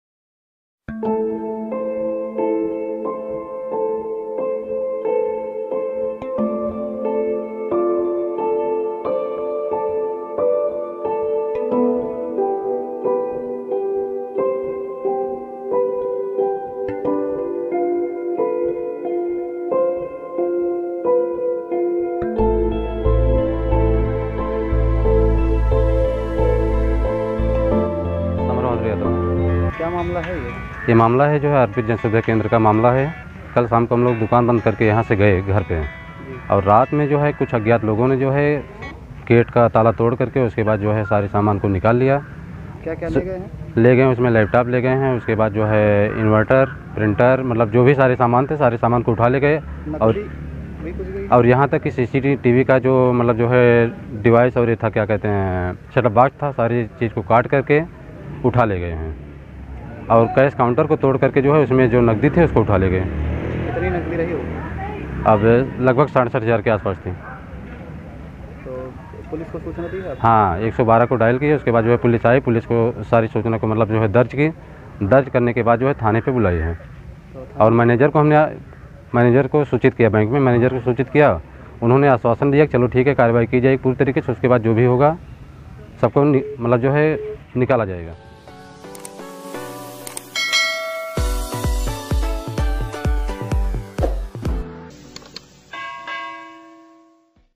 चांदा में हुई ₹67000 की चोरी। सुल्तानपुर न्यूज़। चांदा बाजार न्यूज़।
चांदा के किन्दीपुर में ग्रामीण बैंक सेवा केंद्र चोरों ने किया हाथ साफ
                   सुल्तानपुर जनपद के चांदा कोतवाली क्षेत्र के अंतर्गत स्थित केंदी पुर बाजार में एक ग्रामीण बैंक सेवा केंद्र पर चोरों द्वारा बीती रात हाथ साफ करने का मामला सामने आया है। सेवा केंद्र संचालक समर बहादुर यादव के अनुसार चोरों ने कैश काउंटर में रखे ₹67000 की नगदी समेत इनवर्टर प्रिंटर लैपटॉप जैसे कई सामान उठा ले गए। चोरों ने सीसीटीवी कैमरे के साथ साथ उसका बॉक्स भी उठा ले गए। सुबह जब समर बहादुर सेवा केंद्र पहुंचे तो हालात देखकर समझ गए कि सेवा केंद्र में चोरी हुई है। इसके बाद उन्होंने डायल 112 की मदद से जानकारी पुलिस को दी। सूचना मिलते ही पुलिस घटनास्थल पर पहुंची और उपयुक्त कार्यवाही का आश्वासन भी दिया। समर बहादुर के अनुसार इसकी सूचना बैंक के शाखा प्रबंधक को भी दे दी गई है।